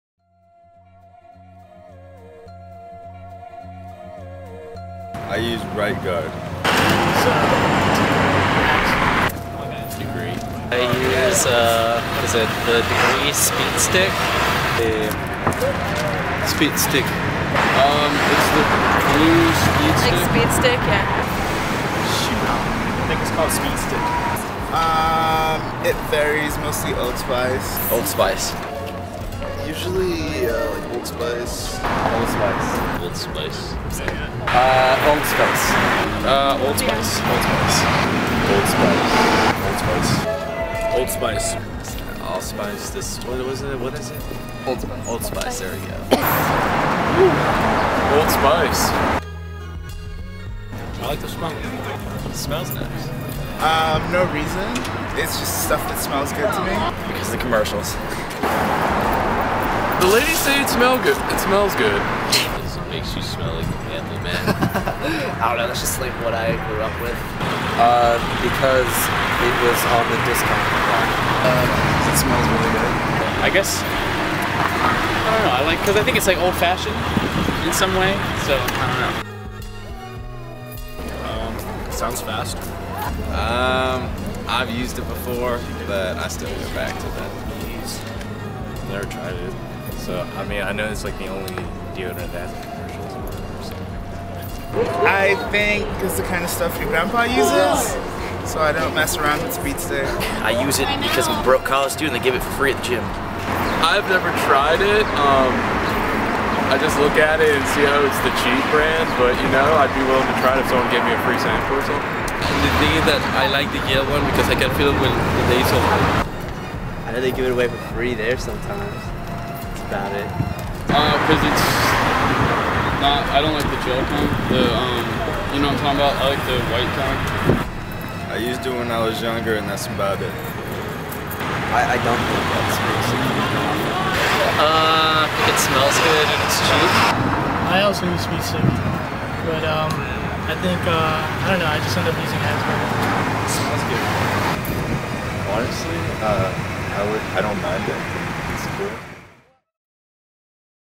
Deodorant survey
Asking around campus about deodorant preferences